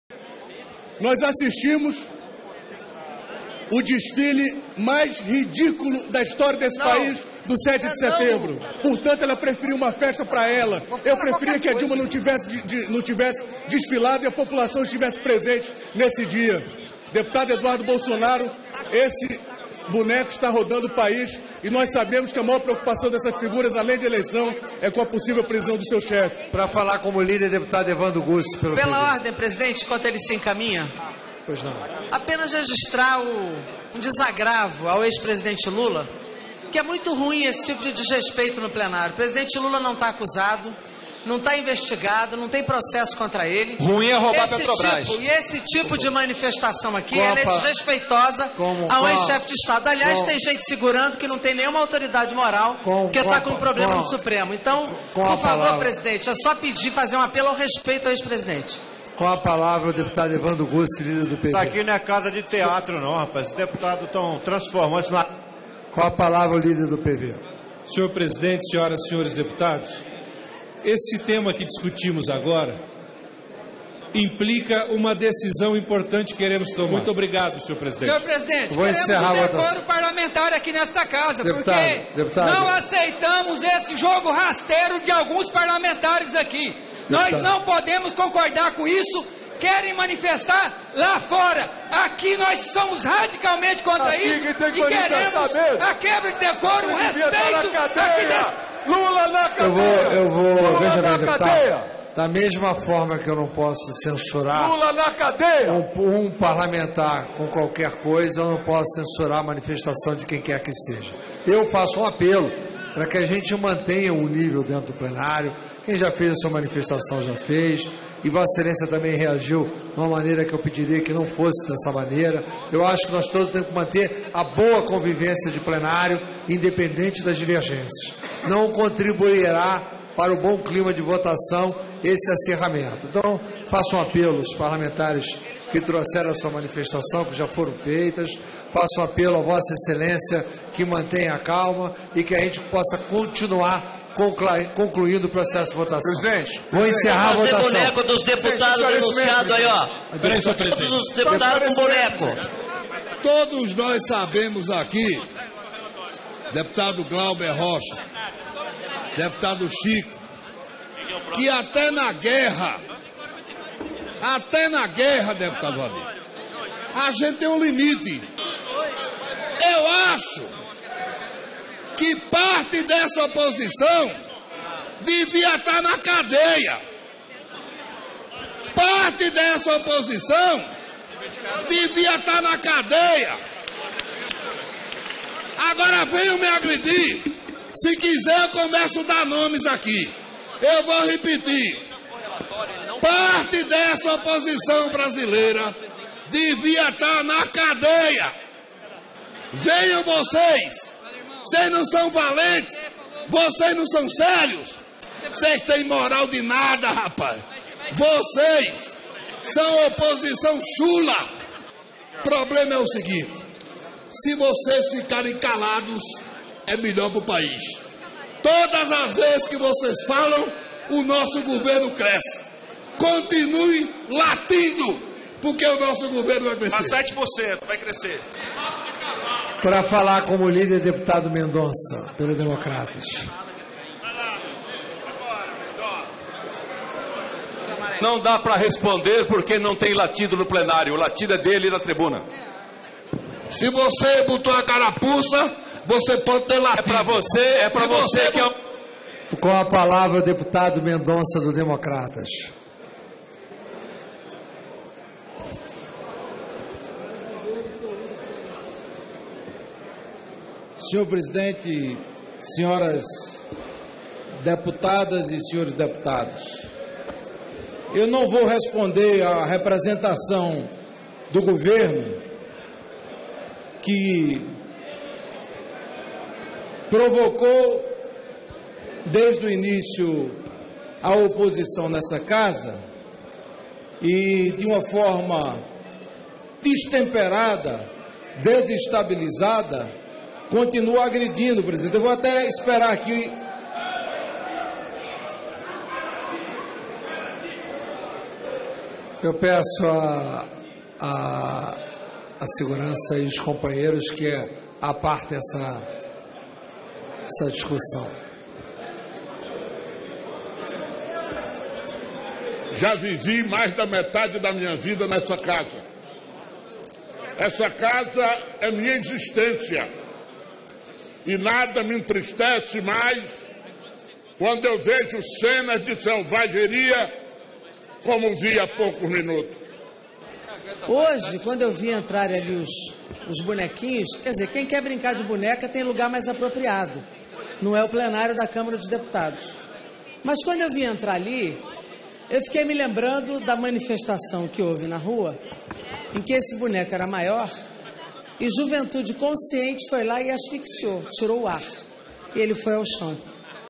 Pixuleco visita Câmara e causa bate-boca
Deputados ultra-petistas - que não são do PT - perderam o equilíbrio após a aparição do Pixuleko, o Lula presidiário, que veio aumentar a bancada do PT na Câmara dos deputados.

Sílvio Costa, deputado-palhaço que é líder do governo (PSC-PE) chamou a oposição para a porrada.

Jandira Feghali, comunista carioca que é cão de guarda do Planalto, louvou a militante da 'juventude esclarecida' que esfaqueou o Pixuleko :

'Quem quer brincar de boneca tem lugar mais apropriado. Eu fiquei me lembrando da manifestação que houve na rua, em que esse boneco era maior, e JUVENTUDE CONSCIENTE foi lá e asfixiou, tirou o ar, e ele foi ao chão'.

Não está fácil para o Professor Cunha manter a ordem nesta Escolinha!

O país se afunda cada vez mais e na mesma sessão foi anunciado o rebaixamento do país pela agência Standard & Poors.

A oposição amanhã começará oficialmente o processo de impeachment de Dilma Rousseff.